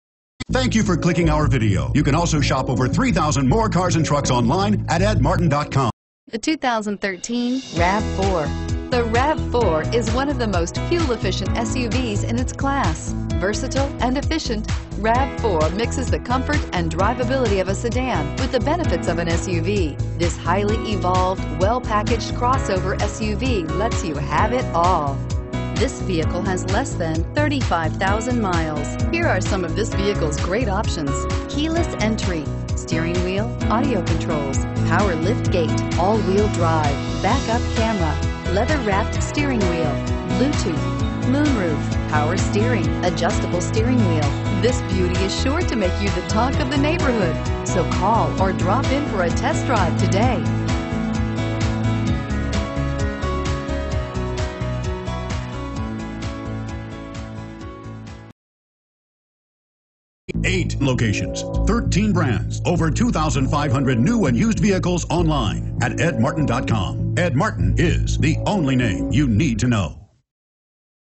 2013 Toyota RAV4 683591A
2013 TOYOTA RAV4 Anderson, IN
Stock# 683591A

Looking for the right car? Today could be your lucky day. With 31527 miles, this Magnetic Gray Metallic, 2013 Toyota RAV4 equipped with Automatic transmission could be yours. Request more information and set up a test drive right away.